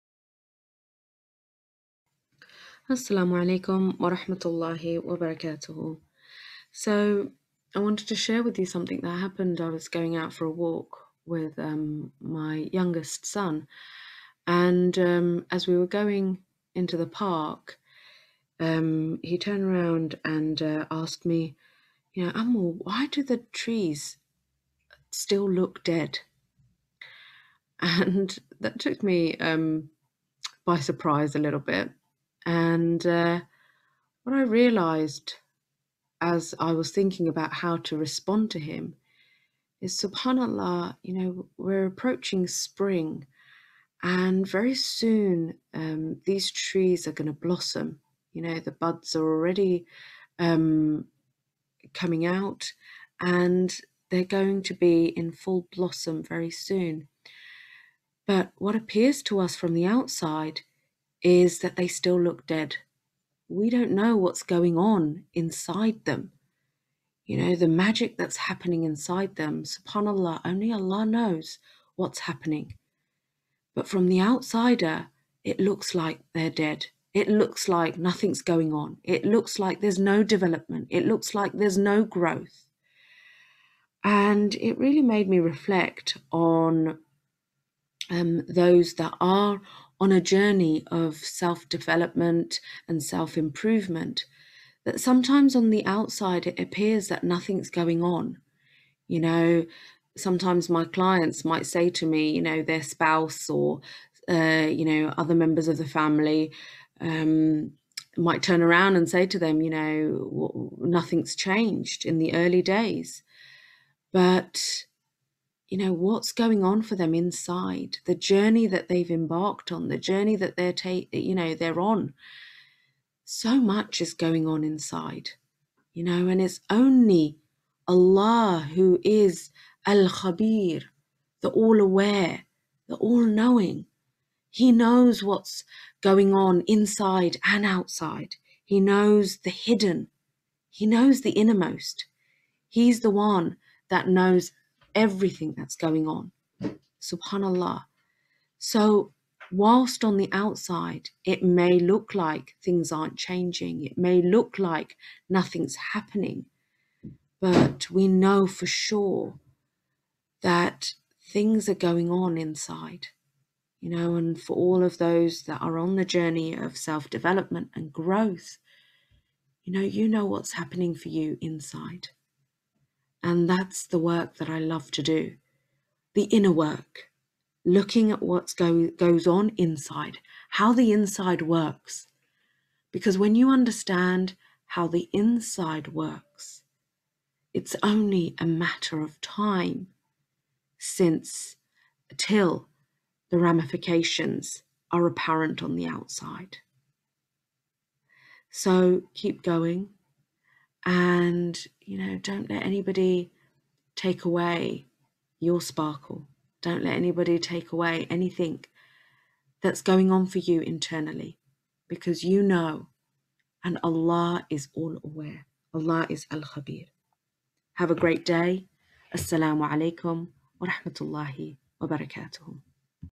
Where growth all starts from- internally!
"Why do the trees look dead mum?"
My son asked me during a recent walk.

I found the question so interesting.

Isn't it amazing how we only see and understand things from the outside and yet don't notice or appreciate what may be going on behind the scenes?

This reflection really helped me connect with Allah's name Al Khabeer.
He knows what is going on inside and out.

Allah says in the Quran He will not change the condition of a person until they change what is within themselves...

What does that mean? Why is looking internally so important? Well what happens with us inside is what manifests outside. It may look like all our problems are due to our external circumstances, but what I love to do with myself and sisters I work with is explore what is going on internally. I appreciate the sisters that I work with and all those that are working hard to explore what's going on for themselves inside.

From that exploration, growth is happening..and Allah is aware of all that you do

Happily yours

Dr Amina